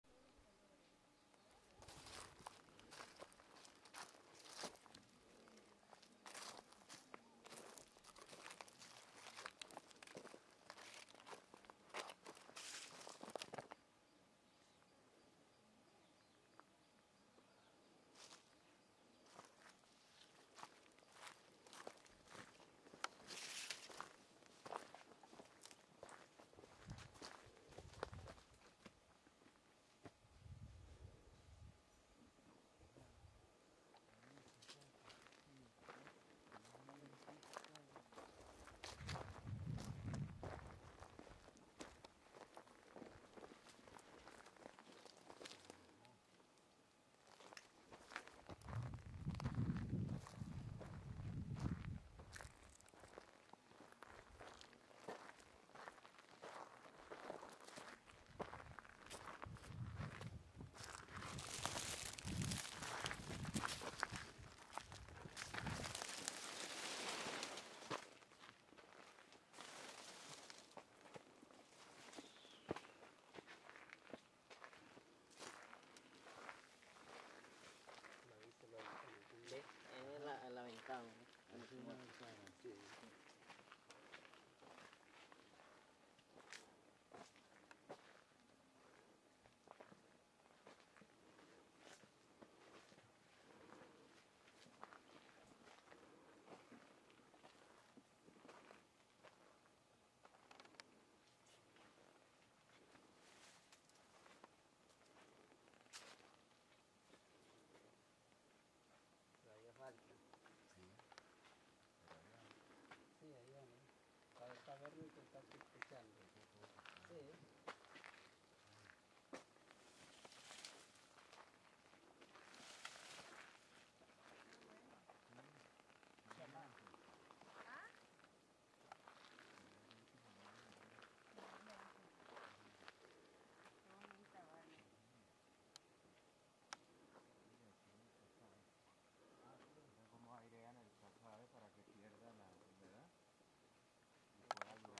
Churuata Piaroa, Estado Amazonas, Venezuela
Vivienda tradicional Piaroa en construcción.
Este tipo de vivienda, conocida como " la catedral de la arquitectura indígena venezolana", está elaborada íntegramente con fibras vegetales por la comunidades Piaroa del río Orinoco según las técnicas tradicionales ancestrales. Actualmente está tipología ha sido prácticamente abandonada.